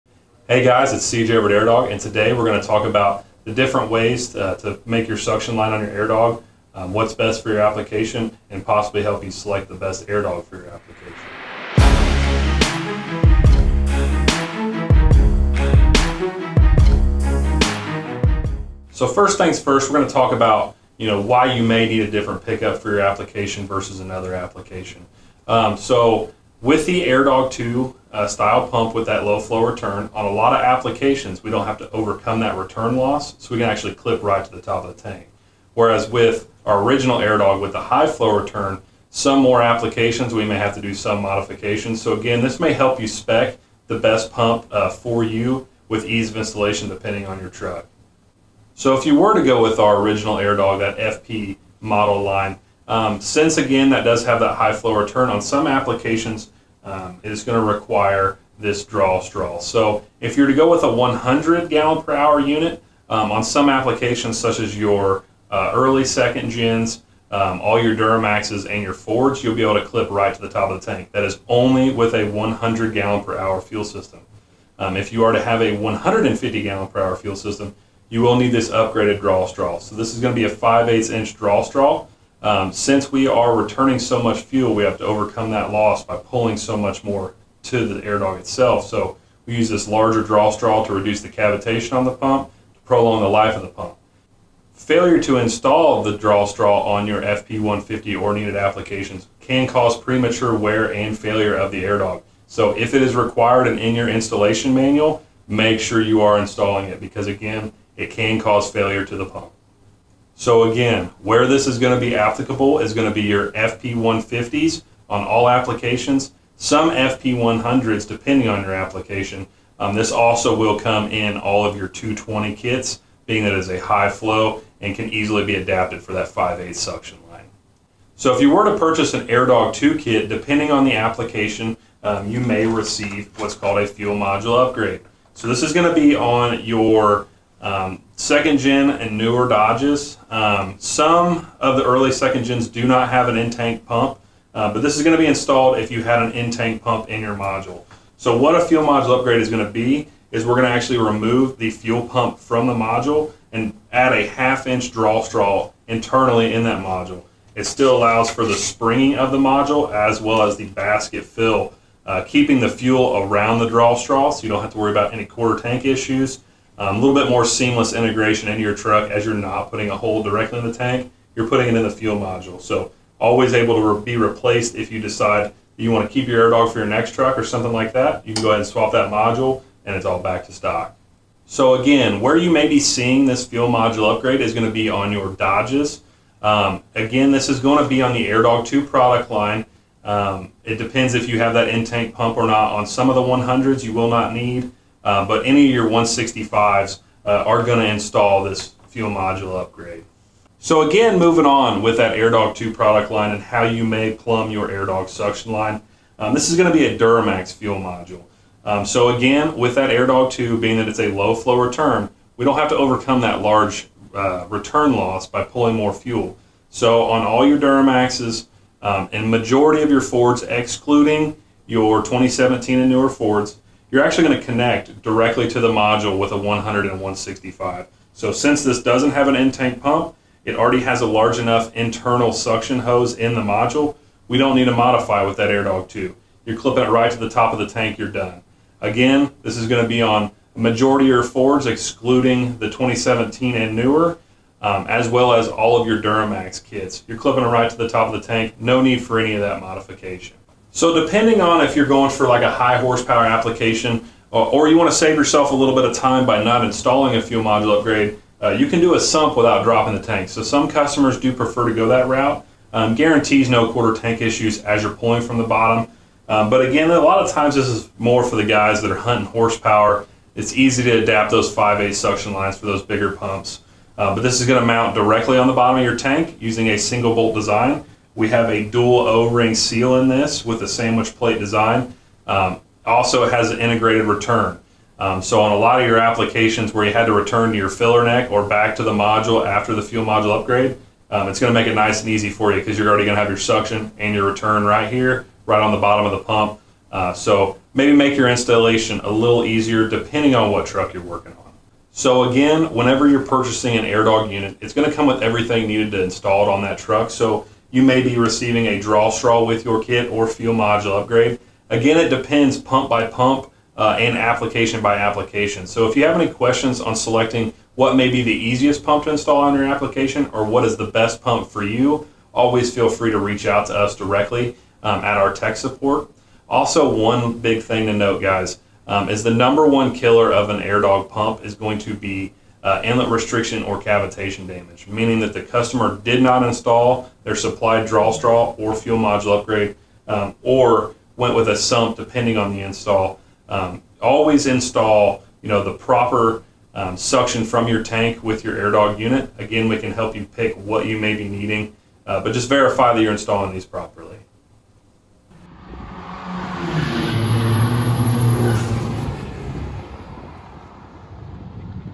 What Suction Line Do You Need For Your AirDog?
There are many different ways to draw fuel from a tank: a high flow suction tube, fuel module upgrade, OEM fuel module, and diesel sump. In this video, we go over what suction line would be best advised for your application and cover how each of these suction lines work. It is crucial to the logevity of your AirDog product that the suction line is a sufficient size for proper flow!